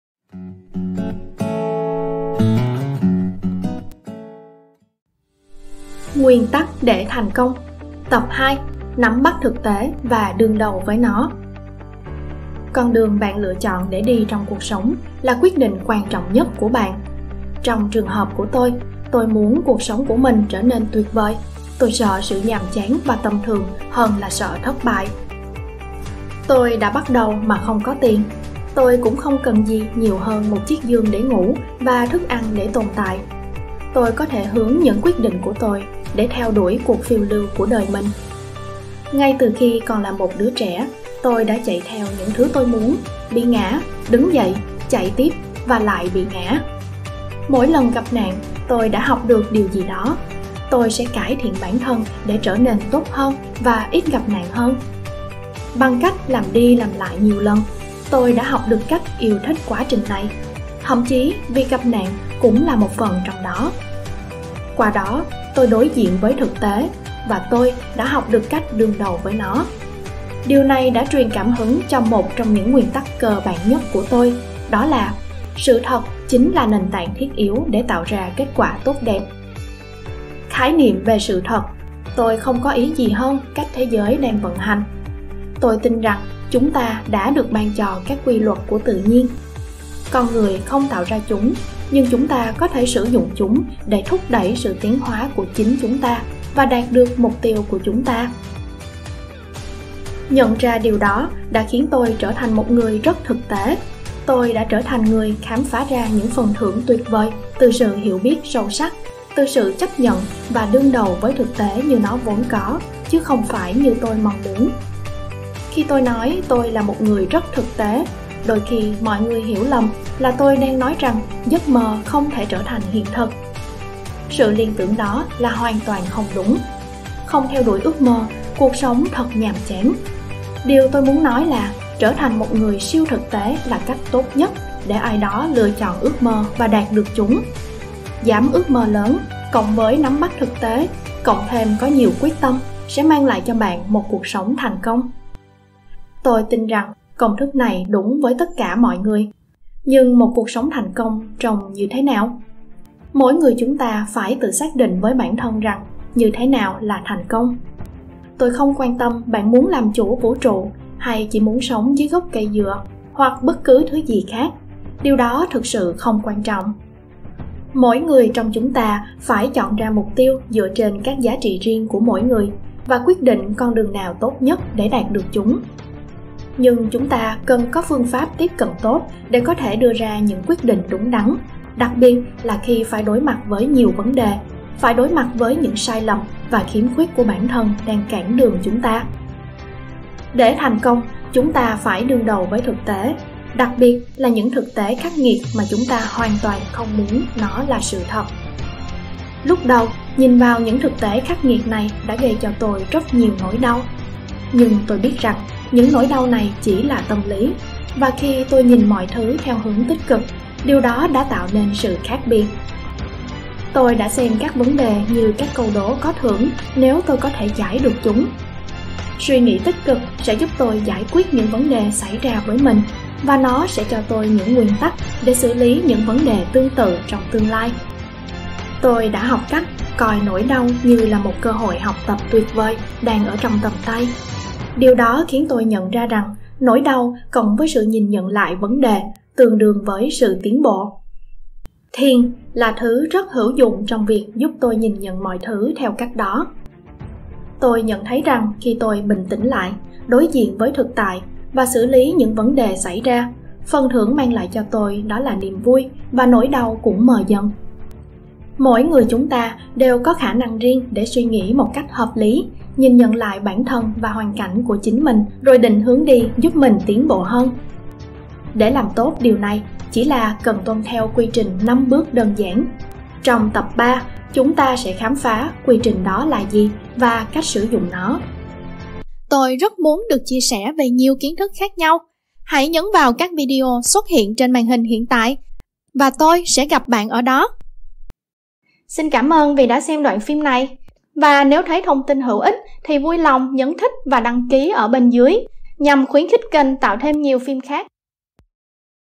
NẮM BẮT THỰC TẠI - NGUYÊN TẮC ĐỂ THÀNH CÔNG - TẬP 2 | Tri thức nhân loại
Trong tập phim này, chúng ta sẽ nghe về tập 2 của cuốn sách nguyên tắc để thành công của Ray Dalio. Nội dung của tập 2 sẽ chủ yếu nói về việc bạn biết nắm bắt thực tại và đương đầu với nó. Đây là cuốn sách được người đọc bình chọn là Bestselling tại New York, Hoa Kỳ.

Xem thêm các video khác của Tri Thức Nhân Loại bằng đường dẫn dưới đây:

Bản quyền thuộc kênh: Tri Thức Nhân Loại